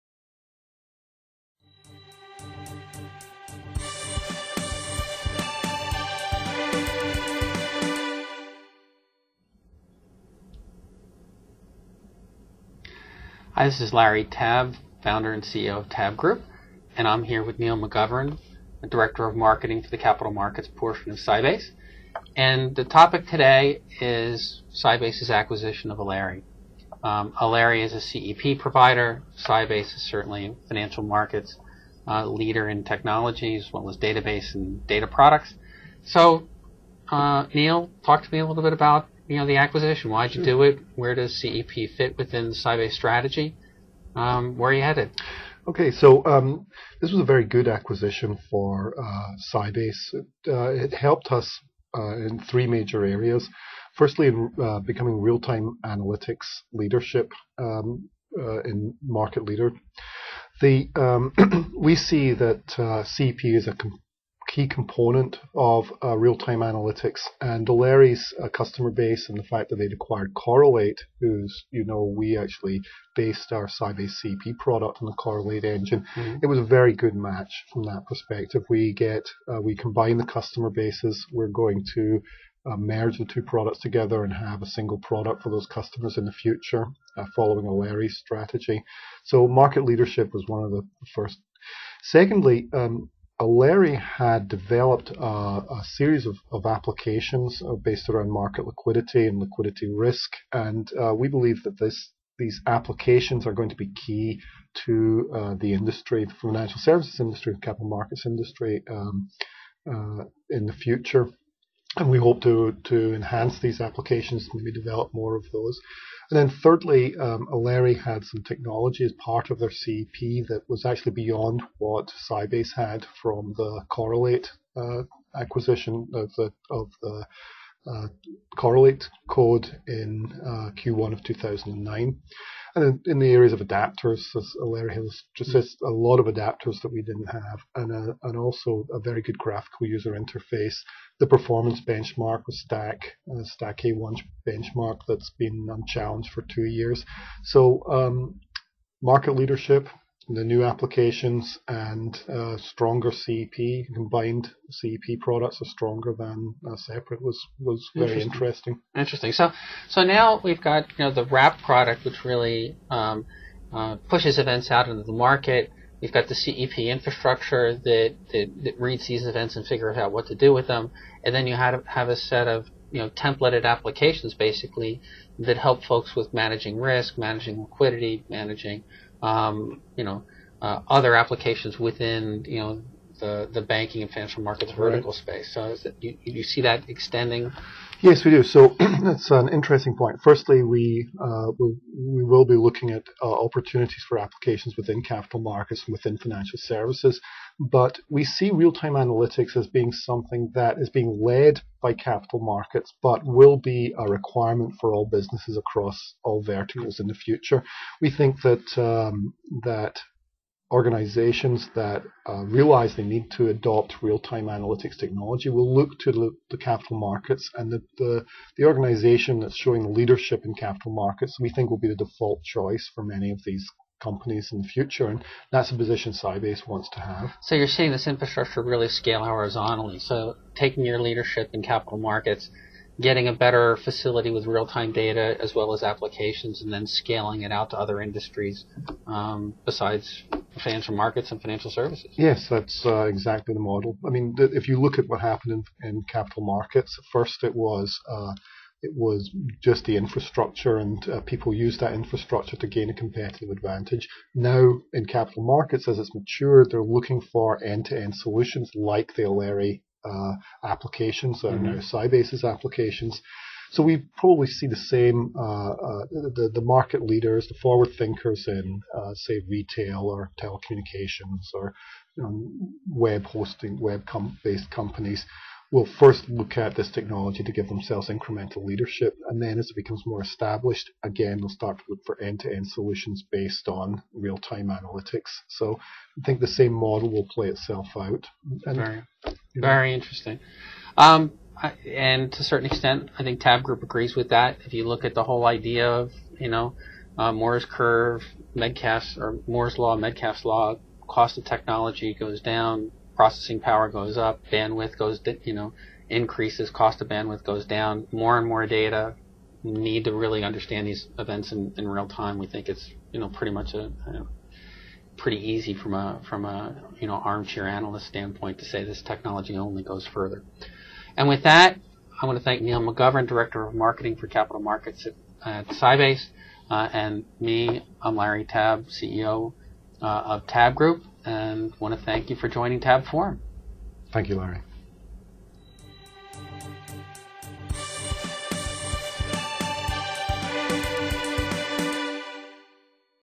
Neil McGovern_Sybase Acquisition Aleri - TABB TV
Neil McGovern of Sybase discusses with Larry Tabb, The Acquisition of Sybase.